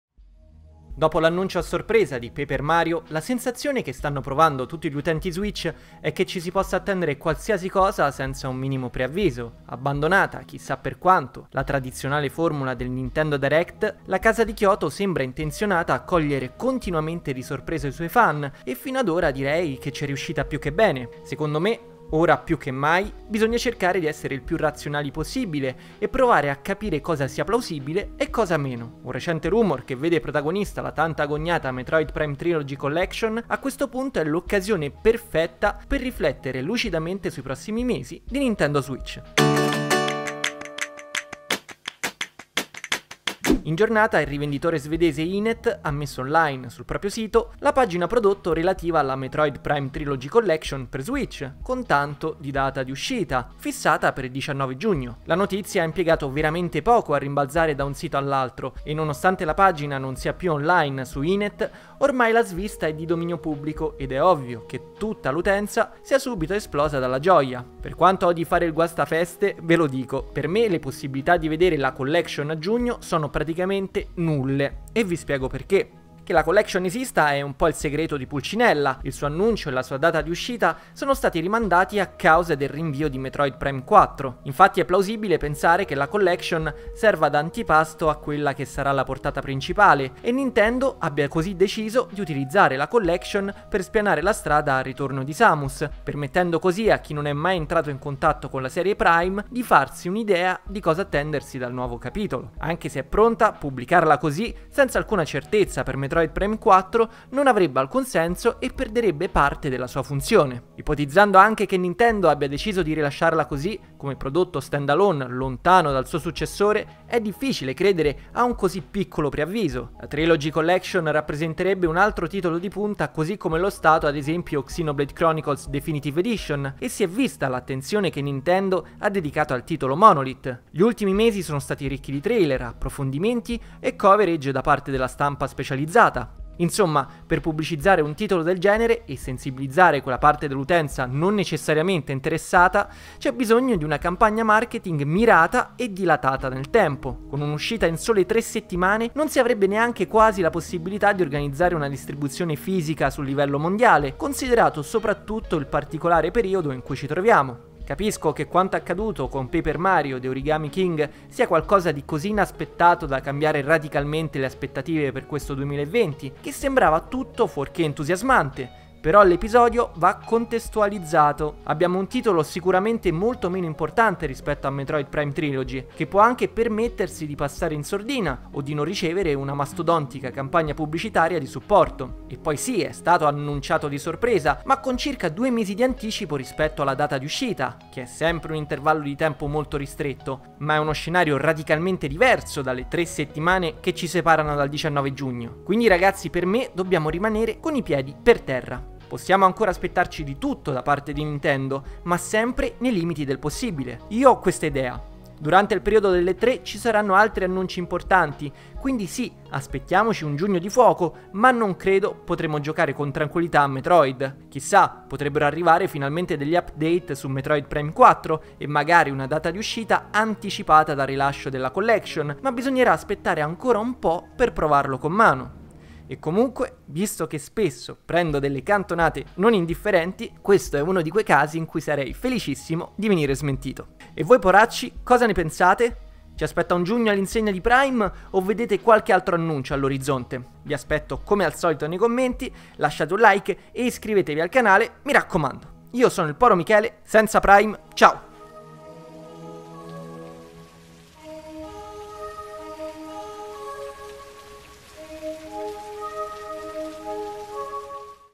[Rumor] Metroid Prime Trilogy Collection a Giugno su Switch? Secondo me...
La Metroid Prime Trilogy Collection torna a far parlare di sè. La tanto attesa remastered su Nintendo Switch è apparsa sul sito di un noto retailer svedese, INET, che ha reso nota l'ipotetica data di lancio. il 19 giugno 2020 - Quanto è plausibile un'uscita così ravvicinata? Veramente mancano meno di tre settimane al ritorno di Samus Aran s Switch, grazie alla Metroid Prime Trilogy Collection?

Io sono il Poro Michele, seza Prime, ciao!